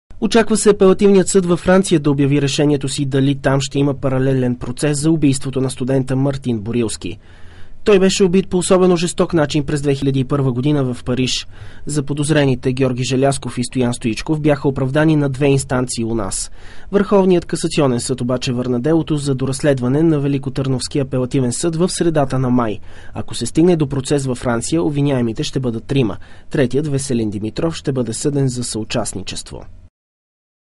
Франция поема делото Борилски
Франция обмисля да повдигне дело срещу убийците на Мартин Борилски, които бяха оправдани от колегите на бащите си в районните ни съдилища.